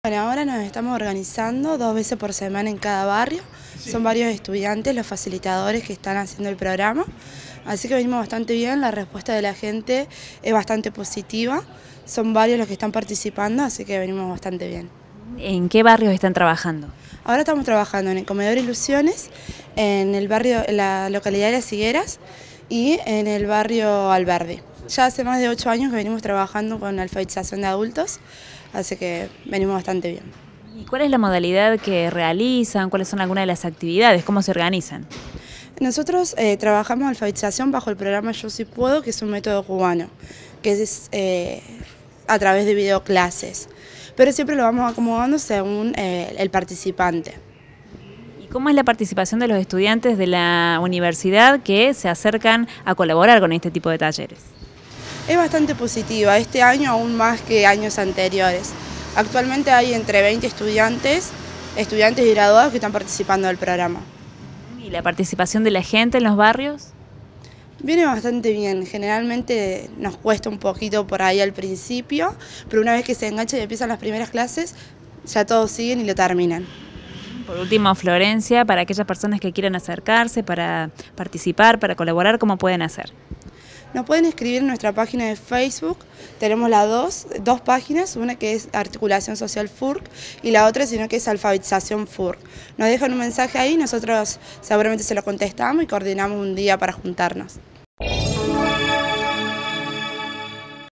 Universidad Informa - Talleres de alfabetización - Florencia Acosta
FLORENCIA ACOSTA – INTEGRANTE FURC. SE REALIZAN ACTIVIDADES EN DISTINTOS PUNTOS DE LA CIUDAD.

Facebook.com/uniriotv
Twitter.com/uniriotv
Instagram.com/uniriotv
Web - uniriotv.unrc.edu.ar
o envianos un email a
UniRio TV
Universidad Nacional de Río Cuarto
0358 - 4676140